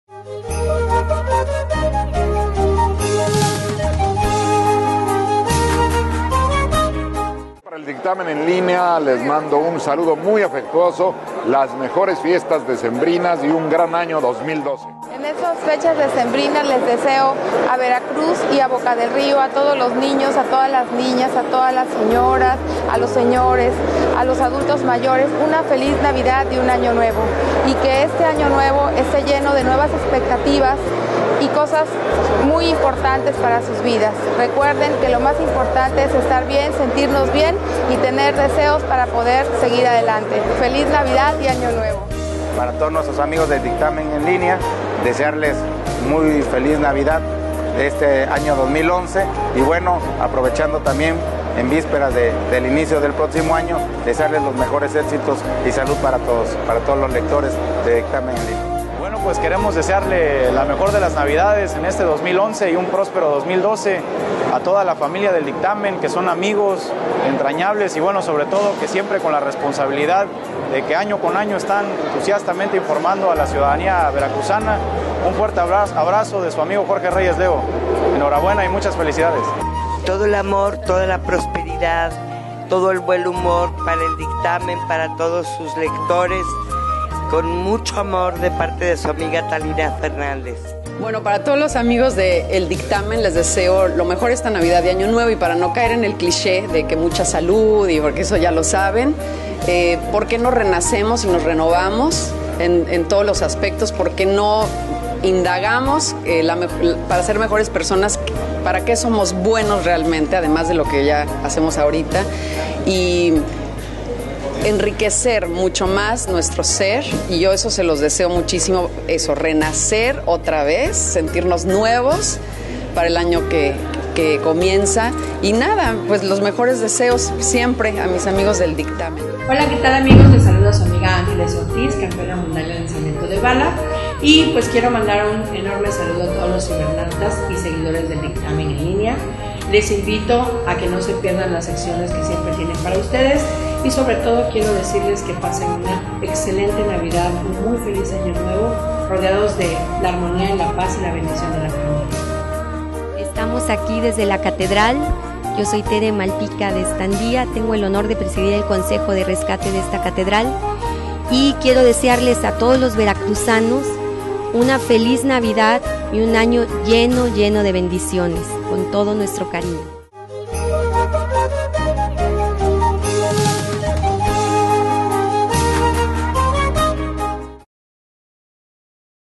Saludos de navidad y prospero 2012 de personajes Veracruzanos II
Los actores políticos y líderes de opinión de Veracruz, desean un prospero año nuevo a todos los habitantes del Estado